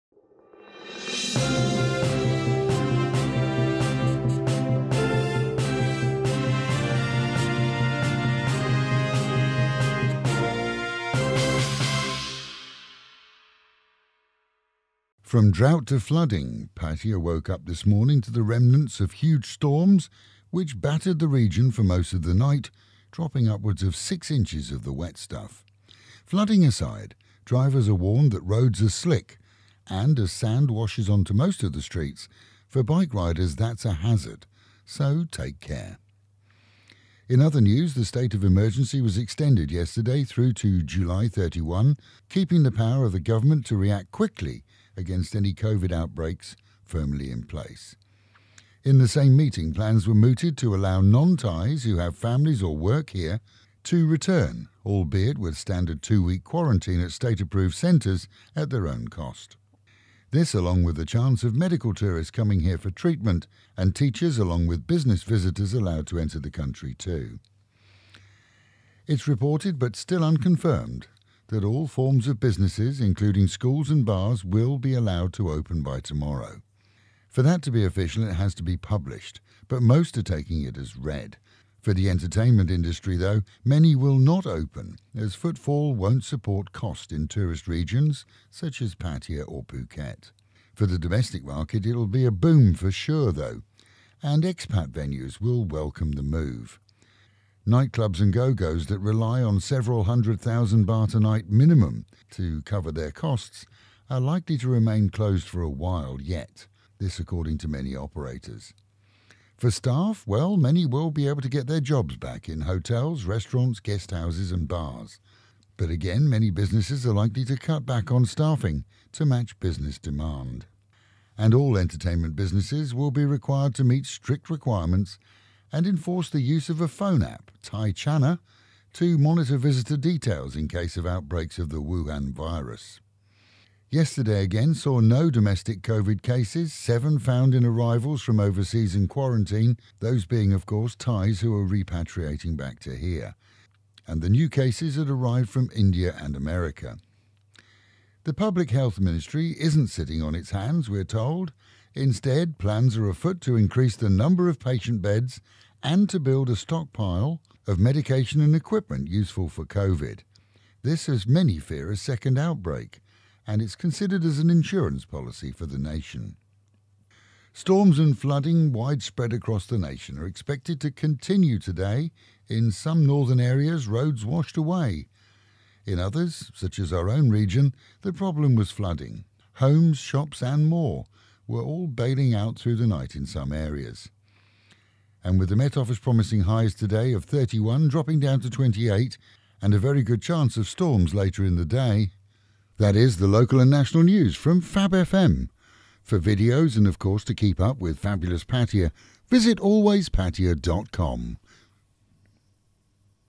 FABULOUS 103FM HEADLINE NEWS from Thailand & Pattaya 30th June 2020
- Covid-19 in Thailand; 0 new domestic cases yesterday, 7 infected returnees in quarantine
- Thailand's State of Emergency extended until July 31st
- Nightlife to resume, some foreigners to be allowed entry to Thailand during Phase 5 of lighter restrictions
- Flooding in Pattaya following heavy storms